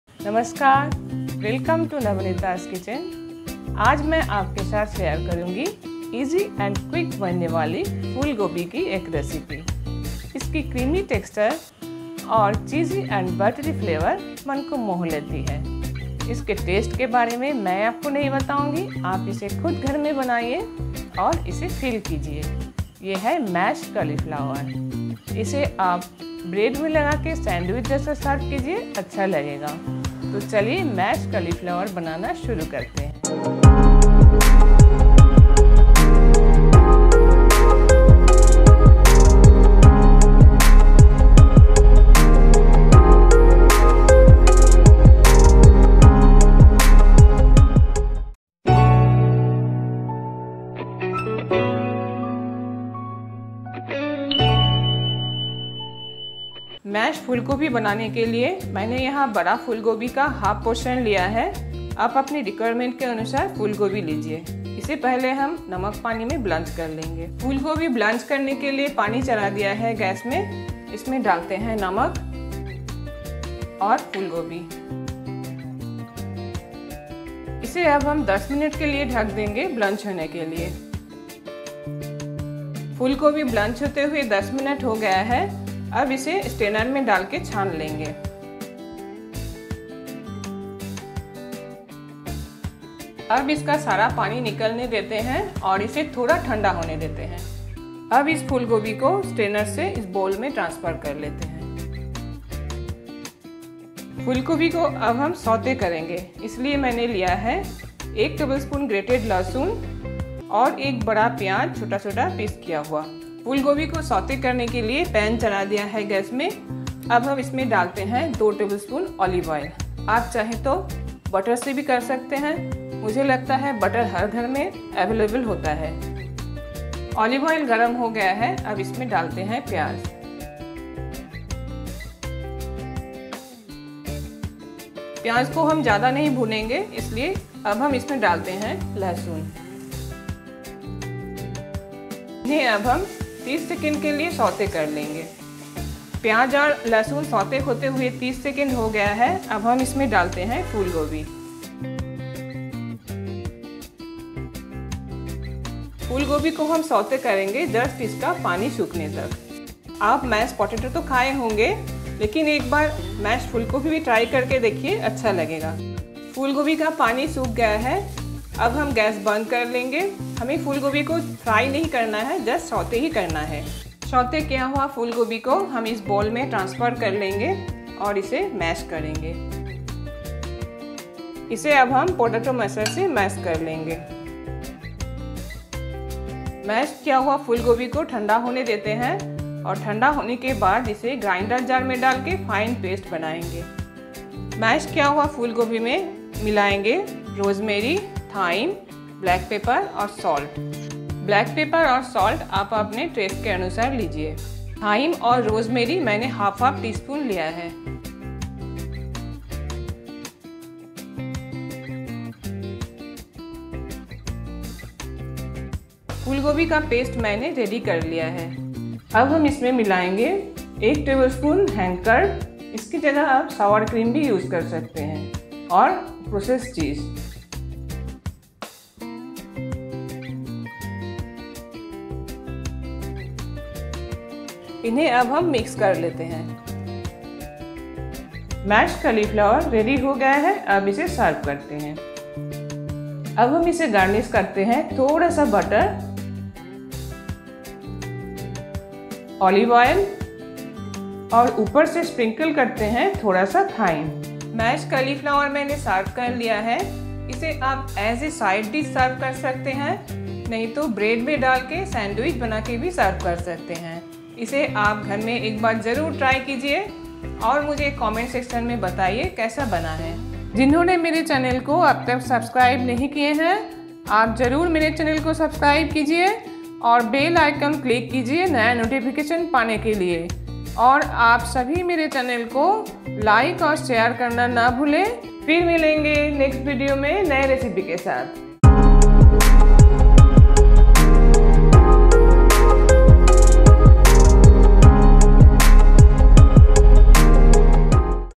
Mashed Cauliflower| Cauliflower Mash | Cauliflower Recipes | Gluten Free | Nabaneeta's Kitchen
Try out this healthy,  low carbs , Gluten free dish Cauliflower mash with herbs and cheese